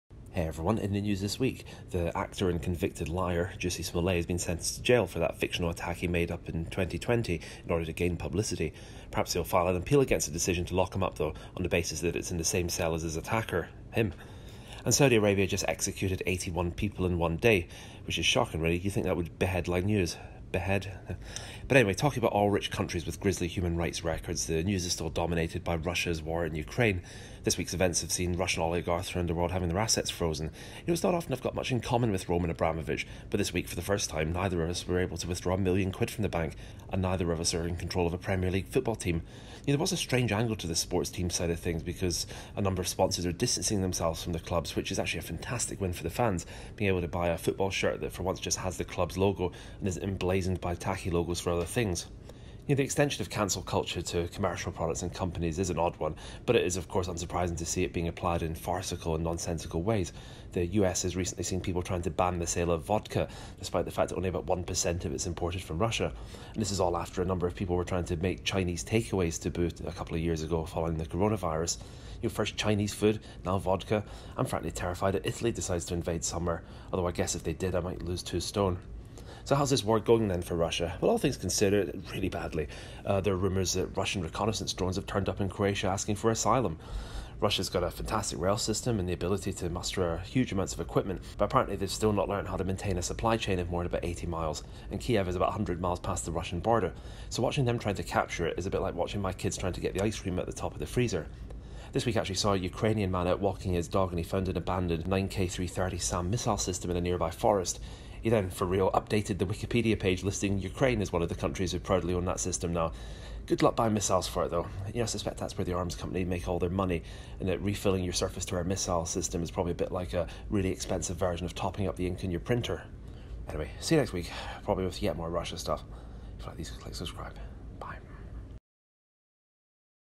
2022 Mar 13 - Ukraine Week 3
News summary for a week in which Russia's invasion of Ukraine continued to go not very well